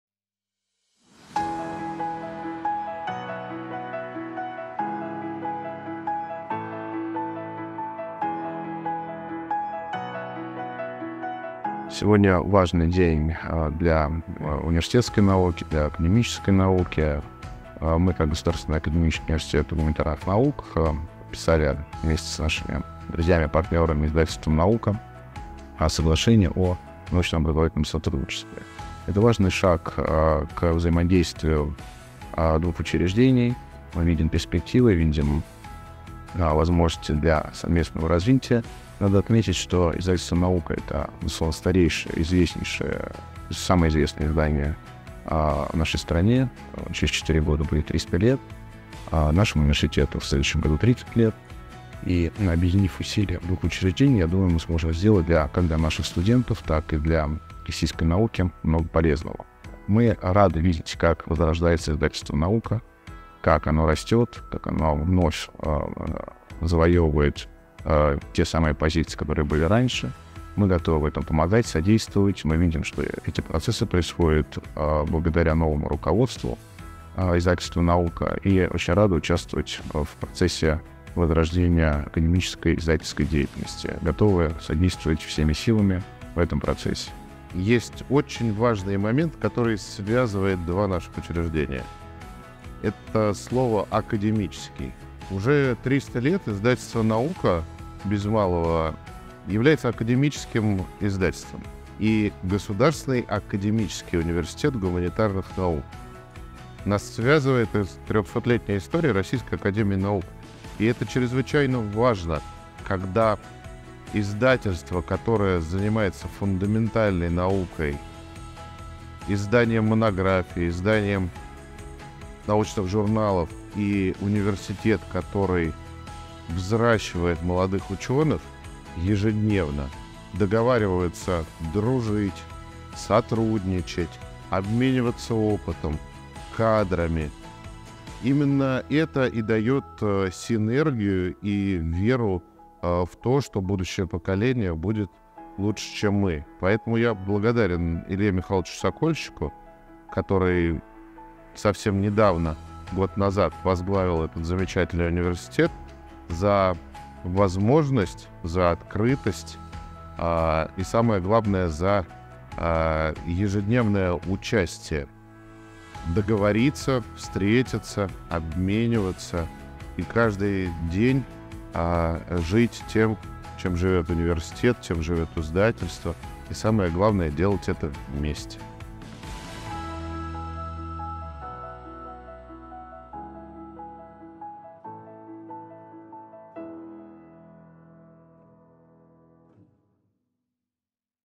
ГАУГН и Издательство «Наука» подписали соглашение о сотрудничестве
14 сентября в Президиуме Российской академии наук Государственный академический университет гуманитарных наук и Издательство «Наука» подписали соглашение о научно-образовательном сотрудничестве.

Директор Издательства «Наука» Николай Федосеенков и врио ректора ГАУГН Илья Сокольщик договорились о долговременном сотрудничестве, в рамках которого будут реализованы программы практической подготовки и стажировок, повышения квалификации для специалистов, проведения совместных культурно-просветительских, научных и образовательных мероприятий для студентов ГАУГН и молодых учёных.

ГАУГН и Издательство «Наука» заинтересованы в подготовке специалистов социогуманитарной направленности в области науки, поэтому каждая сторона выразила свою готовность в создании благоприятных условий для развития совместной научно-образовательной среды.
«Подписание соглашения — это важный шаг к взаимодействию учреждений с перспективами совместного развития. Объединив усилия ГАУГН и Издательства «Наука», мы сможем сделать много полезного. Для каждого исследователя всегда была большая честь публиковать свою научную работу в Издательстве «Наука». Мы рады видеть то, как возрождается Издательство, и готовы этому всячески содействовать», — рассказал врио ректора ГАУГН Илья Сокольщик.

«ГАУГН и Издательство «Наука» связывает Российская академия наук. Чрезвычайно важно, когда Издательство «Наука» с почти 300-летней историей и Государственный академический университет гуманитарных наук начинают сотрудничать, обмениваться опытом, кадрами», — отметил директор Издательства «Наука» Николай Федосеенков.